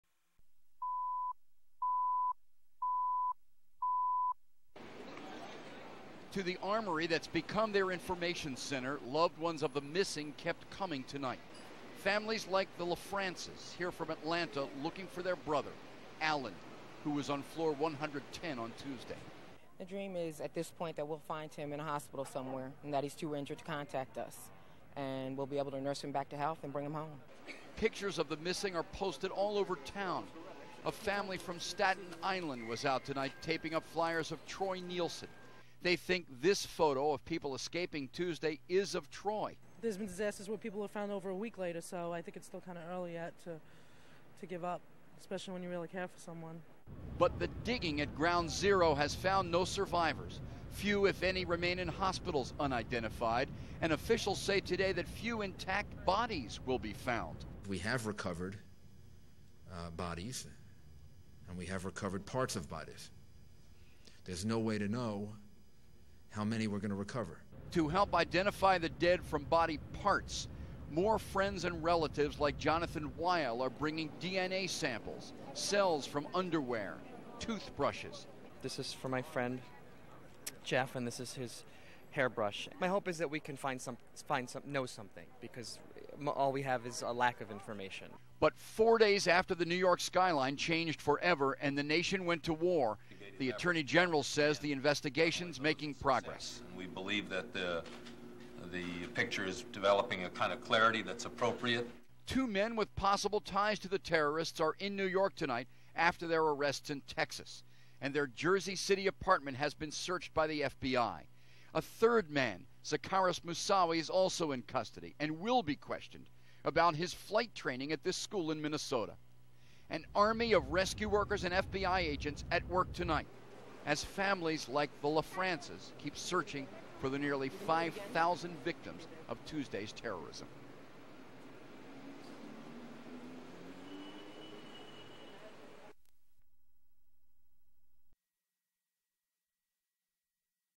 New York 9/15/2001
Video footage from the KXAS-TV/NBC station in Fort Worth, Texas, to accompany a news story.

Physical Description
1 videocassette (DVCPRO) (2 min., 20 sec.) : sd., col. ; 1/4 in.

Created
September 15, 2001, 8:00 p.m.

RIGHTS HOLDER
NBC5/KXAS-TV
Main Title: [News Clip: New York]